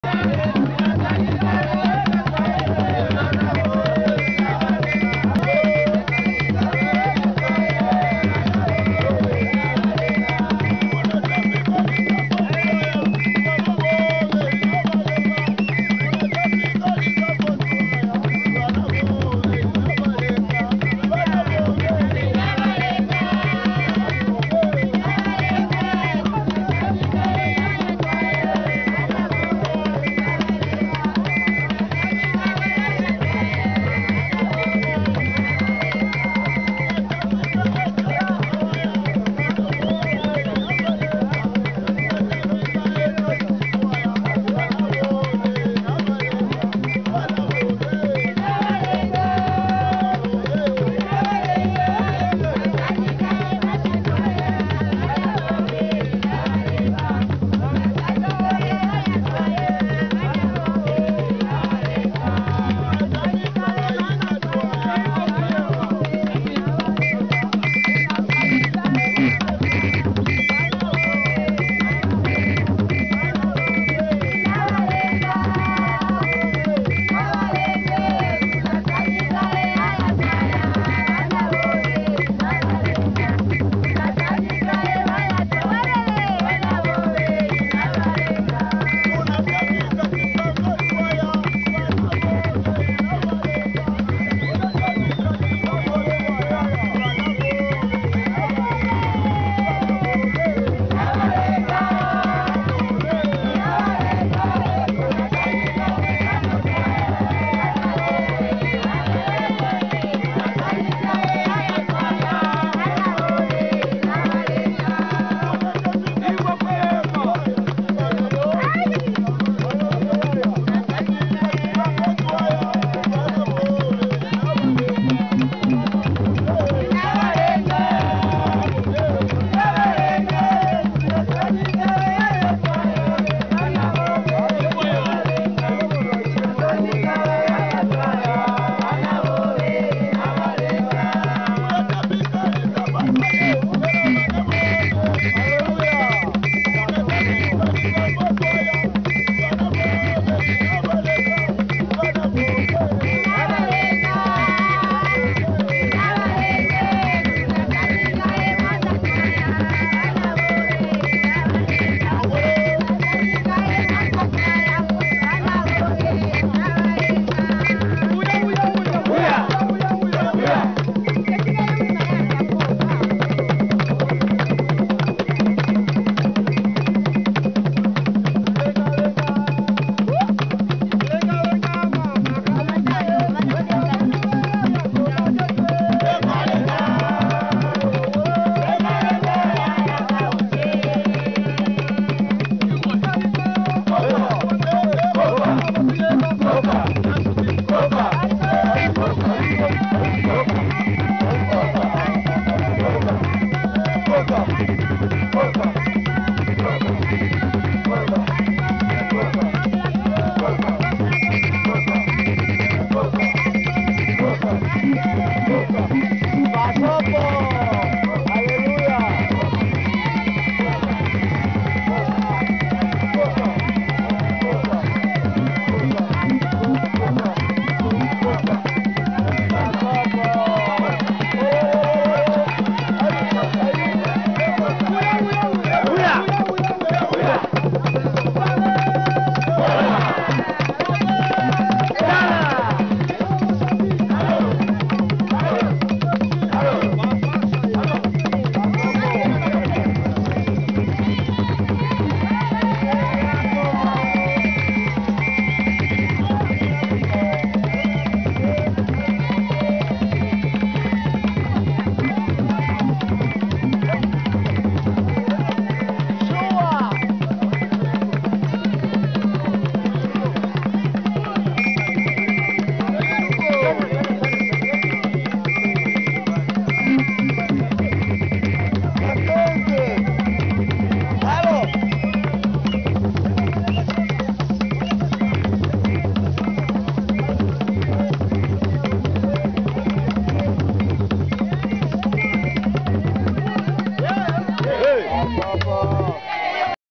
Makishi Dance - Lizambo
Many thanks kuli va muata va Chipango and the family, and everyone for the successful and colourful ceremony at the Chipango's farm in Livingstone.